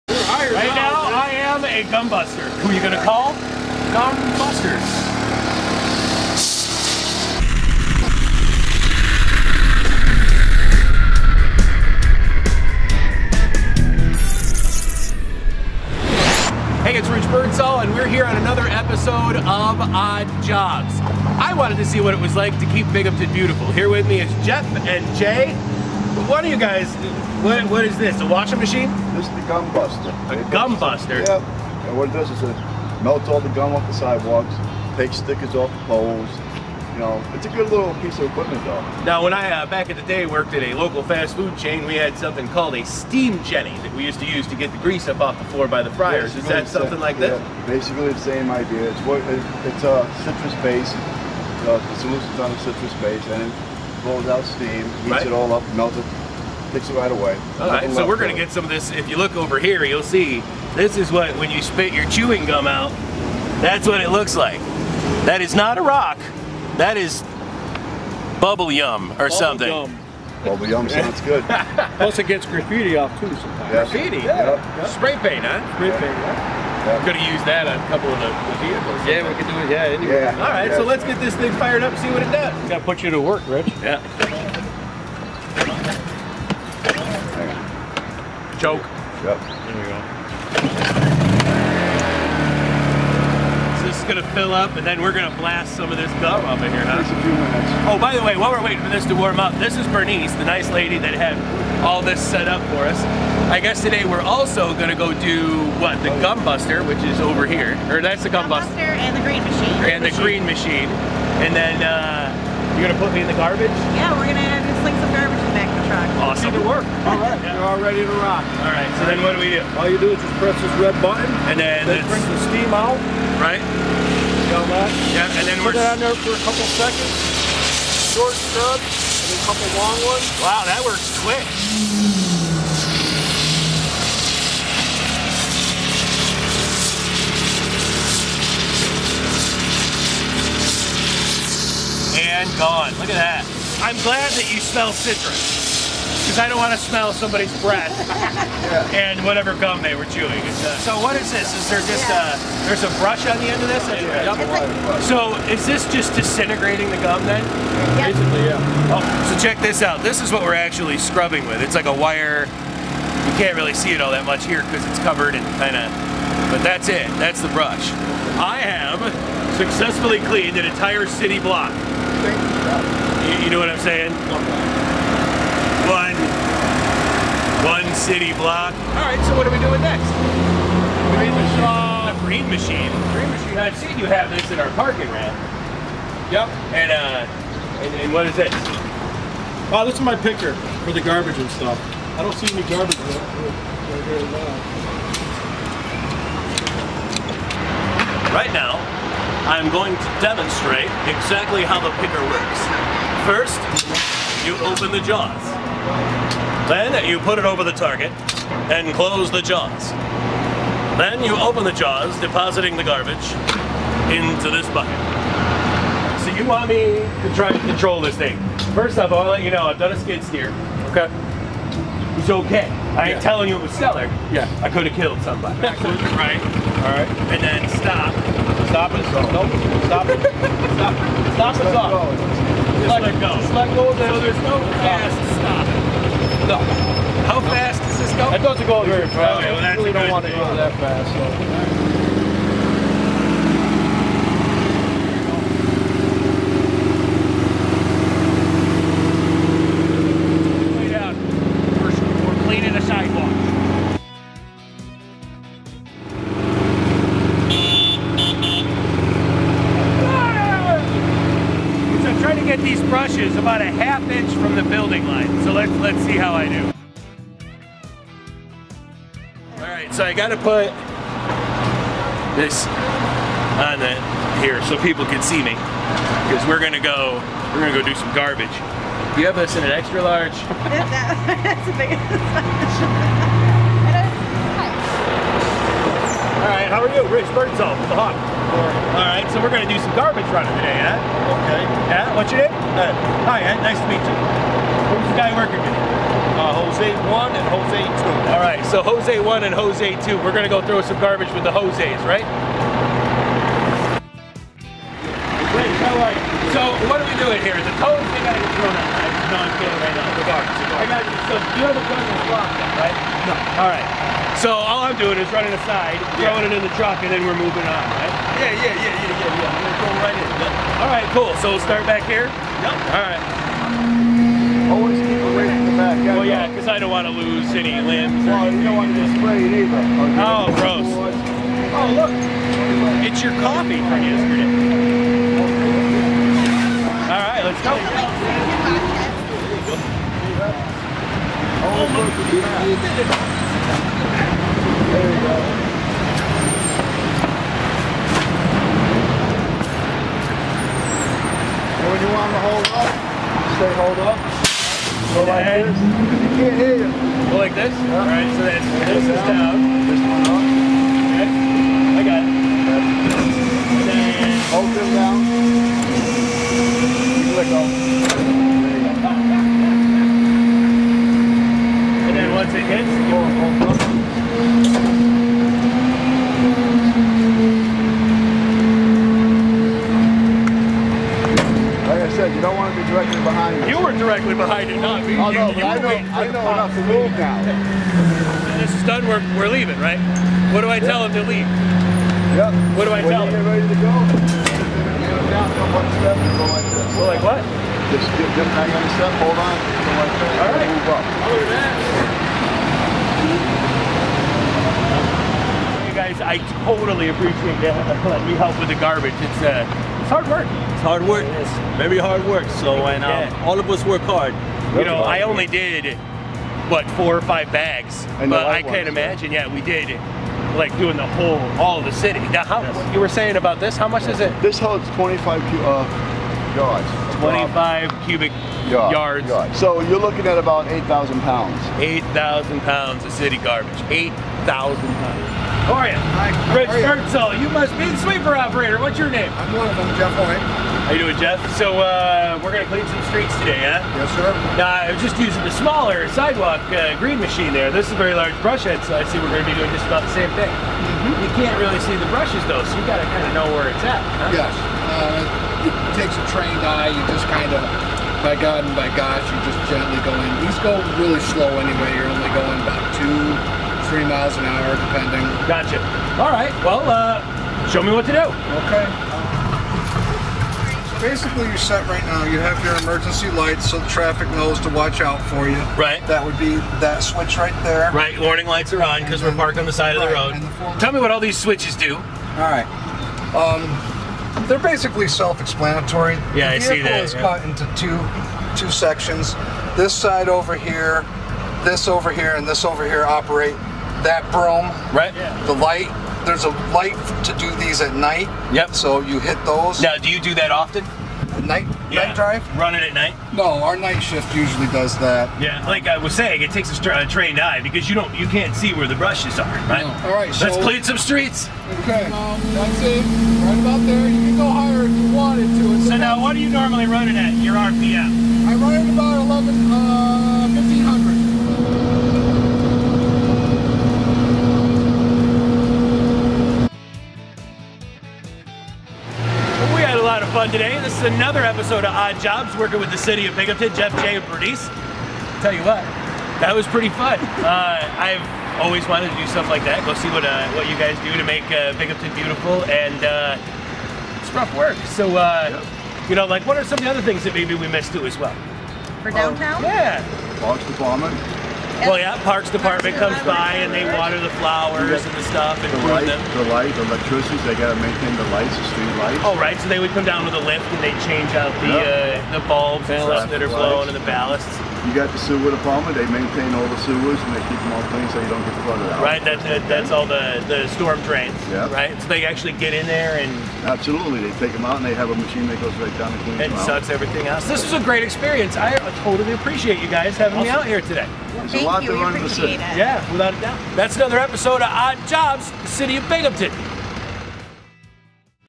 Binghamton DPW Worker - Birdsall's Odd Jobs
We want to let everyone see what you do for a living, your passions, your everyday. So Rich is going out and about in the Southern Tier every week to do your job!